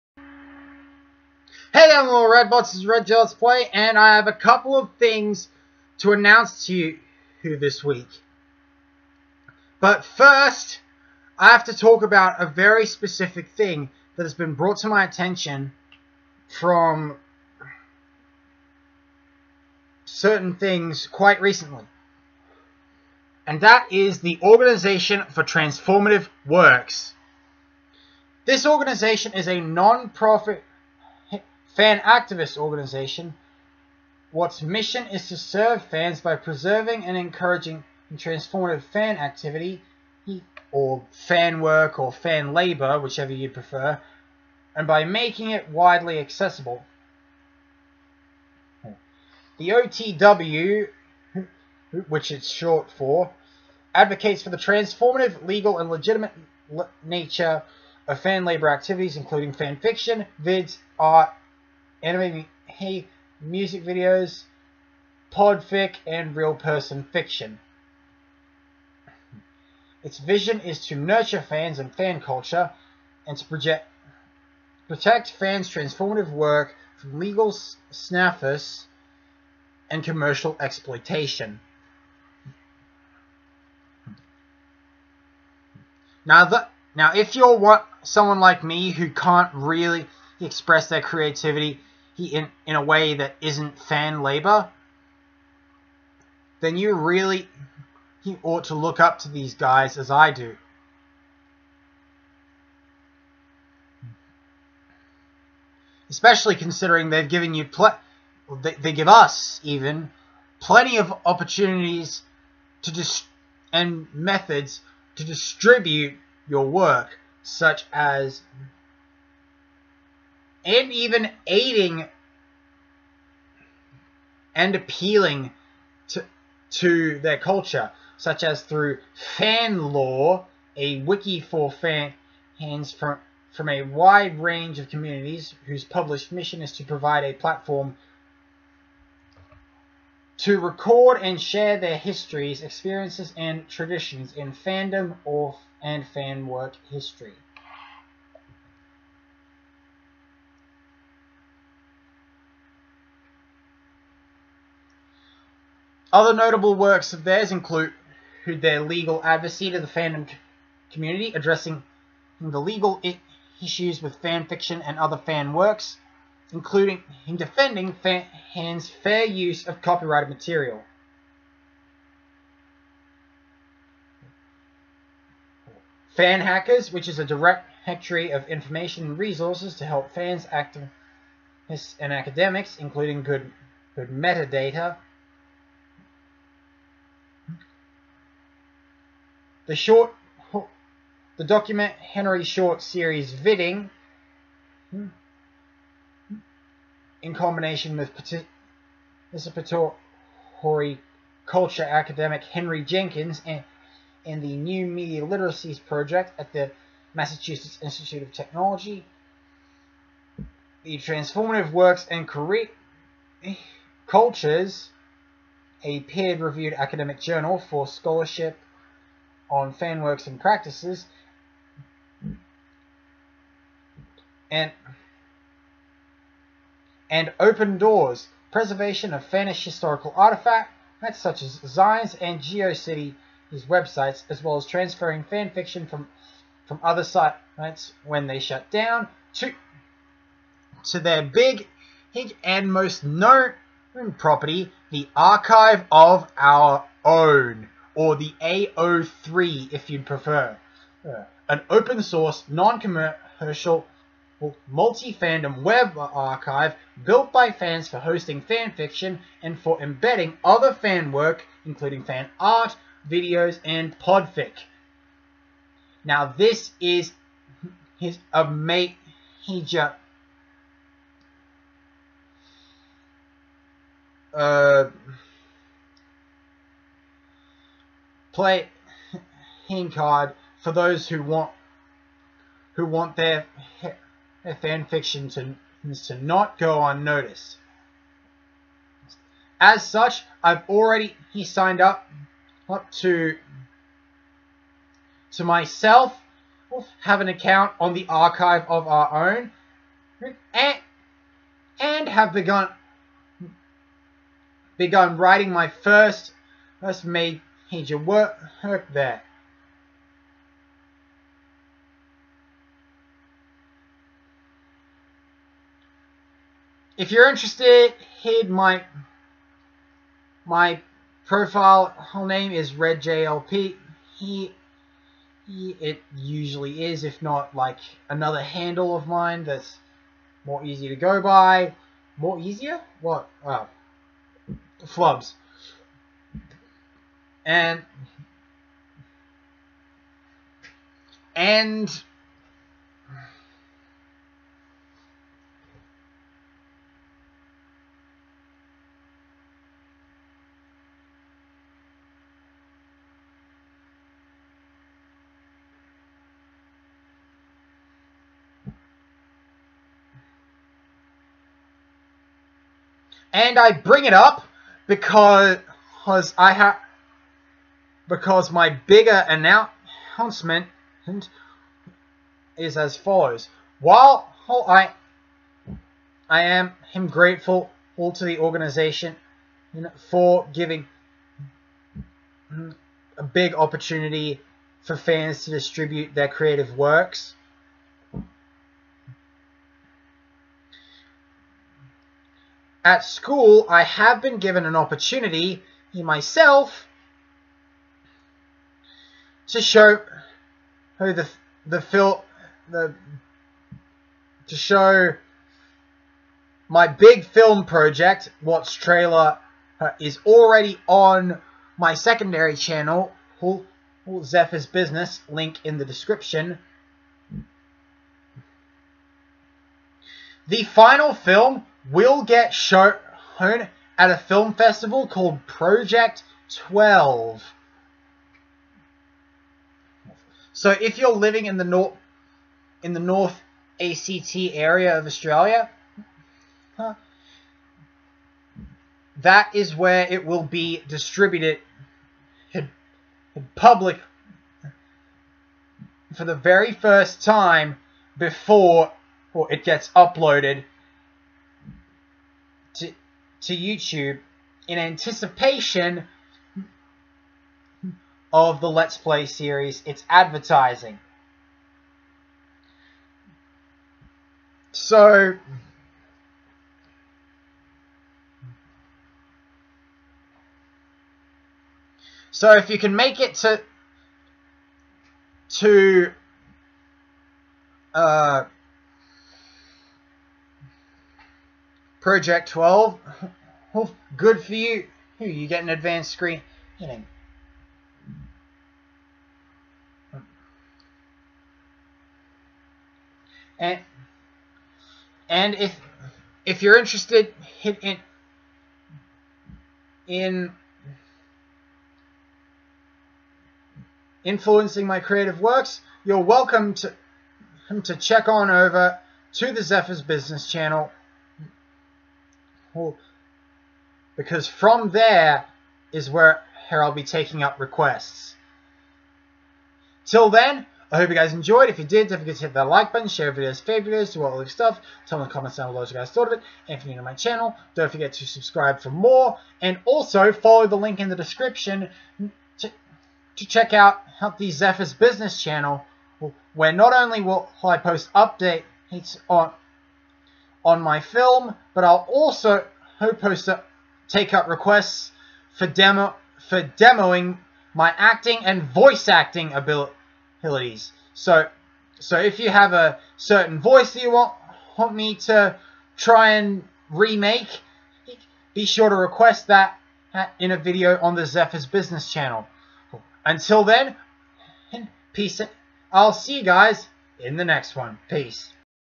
Update: Creative Freedom and Film Distribution
I discuss an Organisation that has founded a website that I'm going to be writing a new story on and then discussing how my film will be distributed.